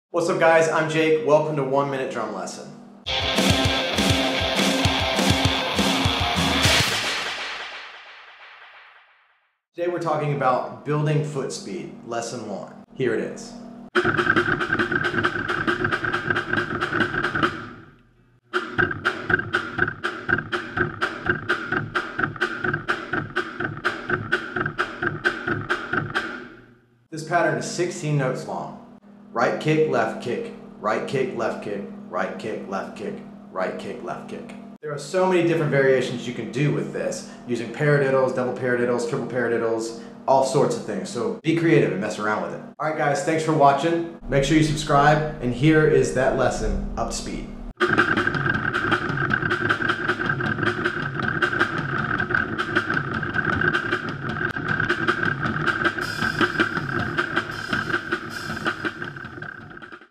One Minute Drum Lesson Building Foot Speed #1 - Jake Bundrick - Mayday Parade
In this first foot speed lesson I'll show you a technique I use to build foot speed

The pattern is: RKLK RKLK RKLK RKLK

Don't just stop there. Try other rudiments like:

The paradiddle: RKLK RKRK LKRK LKLK
Double Paradiddle: RKLK RKLK RKRK LKRK LKRK LKLK
ETC.